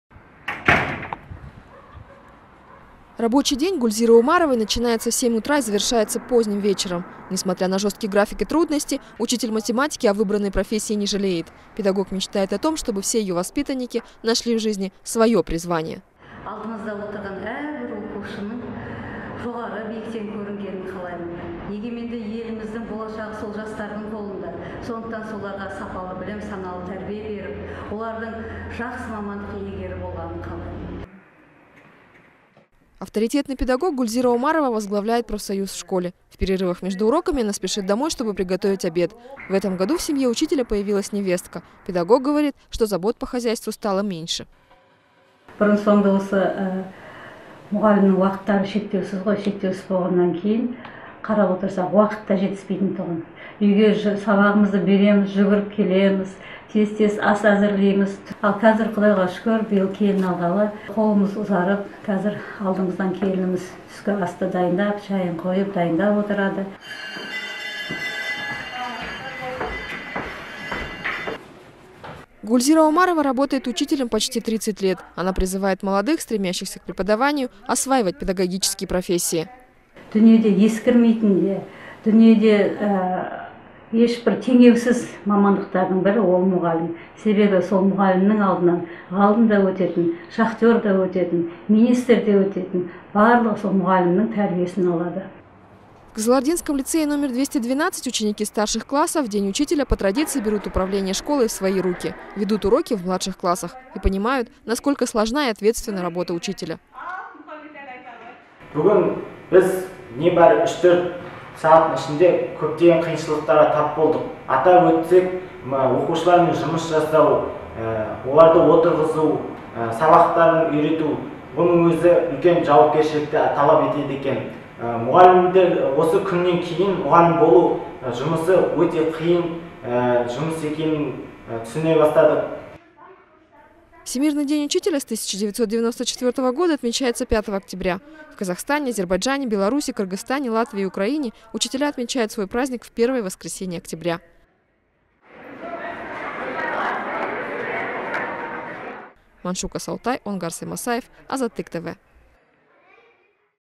Один день из жизни учителя
Учитель математики Гульзира Омарова работает в школе три десятилетия. Лицей № 212 в Кызылорде, где она преподает, за полвека окончило более пяти тысяч человек.